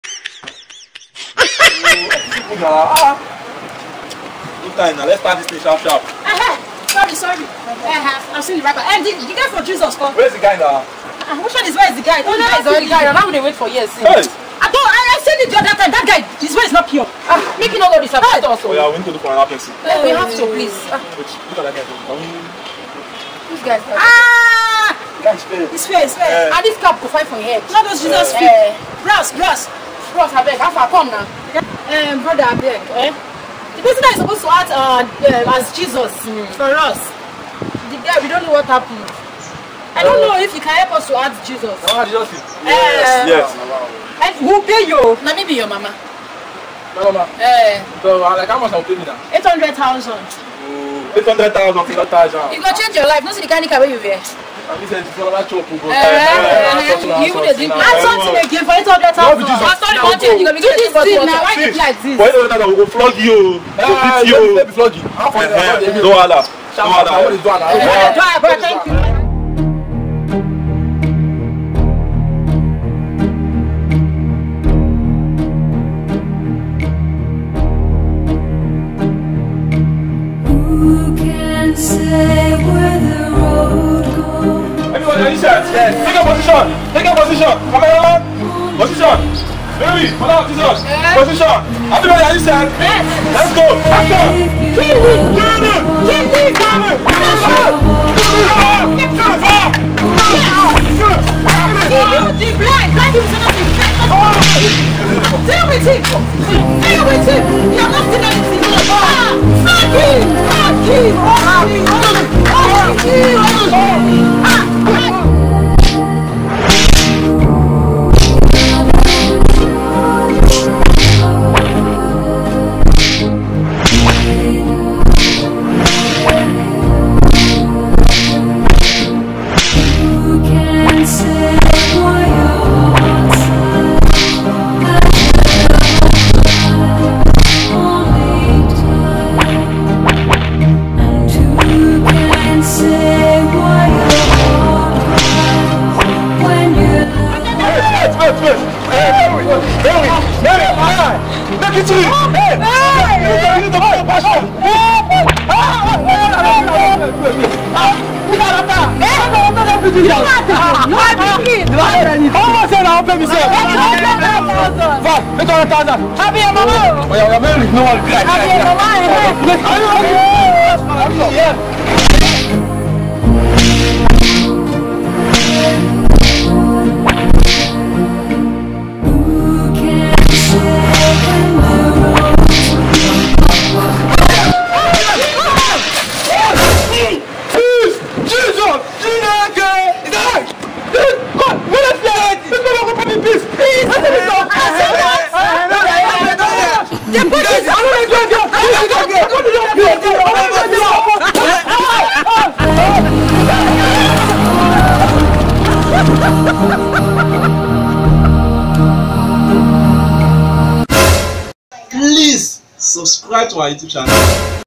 Passion Of Christ (Orobo Noni Comedy)
This video is about the agony of Jesus Christ.
It is not just a comedy video but there's a message for everyone.